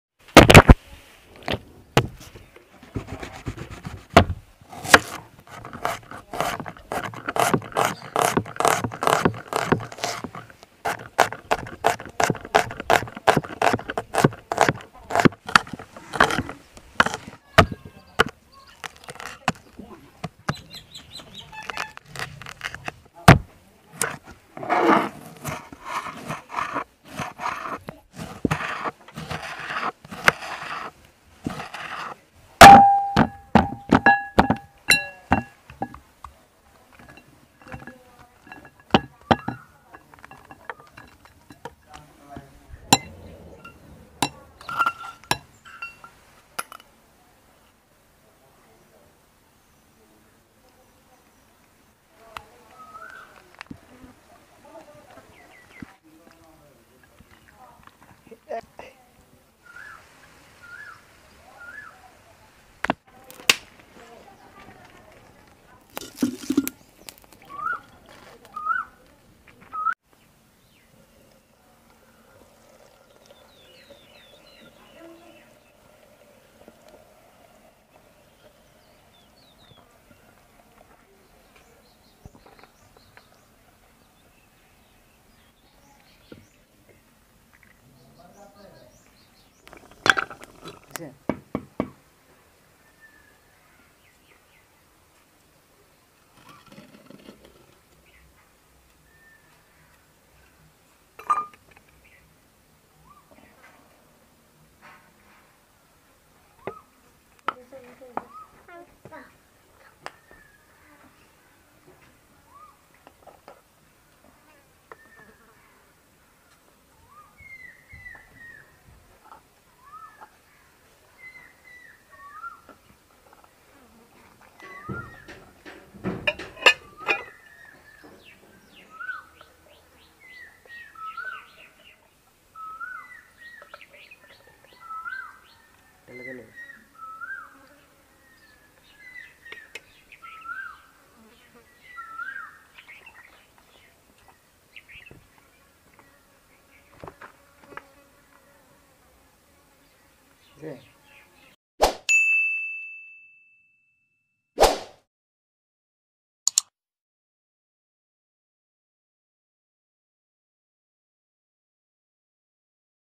HOW TO MAKE CHICKEN HANDI | HANDI CHICKEN RECIPE 🐔
handi chicken recipe ,
meat, minced garlic,
whole garlic, half chopped,
 coriander powder,
 cumin powder,
 green chillies,
 chopped tomatoes,
 turmeric powder, little meat masala, salt to taste, garam masala,
 sour curd, refined oil,
 mixed everything, then Heat oil in a clay pot and mix it with bay leaf,
cinnamon and add the meatI gave it and then covered the face well with a clay pot and let it breathe for 30 minutes.
This is how our handi chicken was made 🐔.